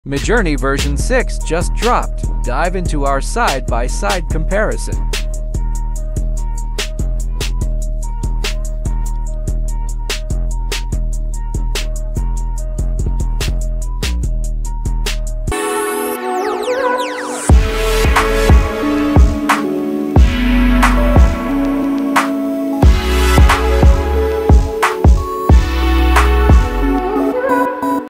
New: Comparison of Midjourney v5.2 vs v6
Here are some new exciting features of Midjourney v6

- MJ6 now understands prompts in everyday language

- Adding text in images

- Expanded prompt length: Over 350 words

- Precise object positioning, like 'the top-right corner'

- Detailed color and feature customization

- Multi-subject prompts for richer images

- Ability to create comics

- Add image frame using descriptive prompt